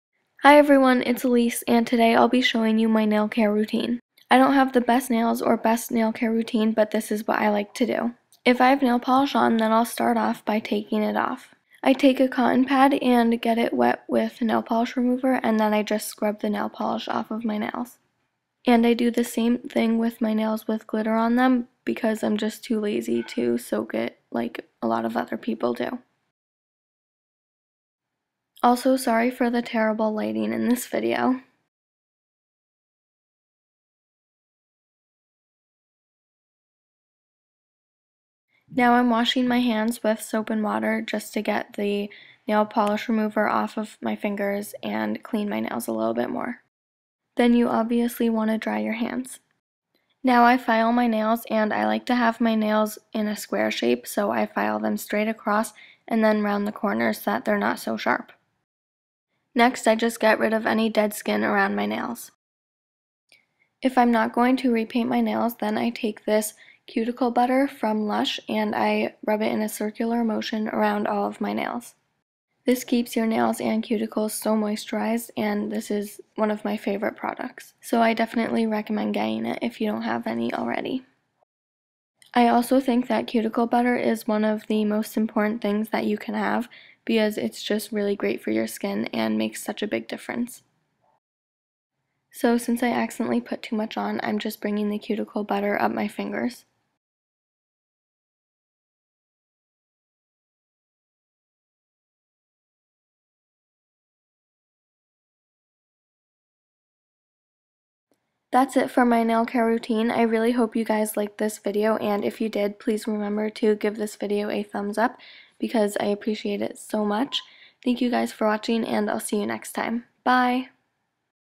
My Nail Care Routine | Swiftpolish
I've gotten a bunch of requests to do a nail care routine video so here it finally is!! The lighting and quality are both terrible, SORRY! If you have any questions ask and I'll reply soon.

The design I wore in this video is a Christmas ornament design but I couldn't get a good picture (due to lighting) and didn't like them enough to post a bad quality picture.